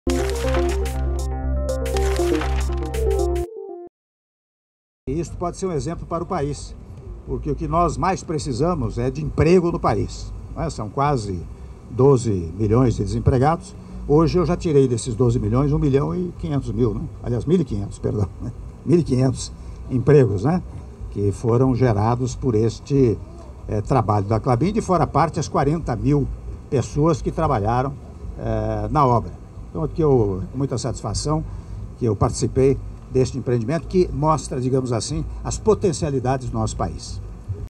Presidente Temer fala sobre criação de empregos
EM DESTAQUE - 28.06.16: Nesta terça-feira, o presidente em exercício Michel Temer participou da inauguração de fábrica de celulose da Klabin, em Ortigueira (PR). A fábrica teve apoio do governo federal por meio de financiamento do Banco Nacional do Desenvolvimento Econômico e Social (‎BNDES)‬. O empreendimento, no valor de R$ 8,5 bilhões, representa o maior investimento privado da história no Paraná e gerou 1.500 novos empregos diretos e indiretos. Na cerimônia de inauguração, o presidente em exercício ressaltou o papel da iniciativa privada para o crescimento do país e na geração de emprego.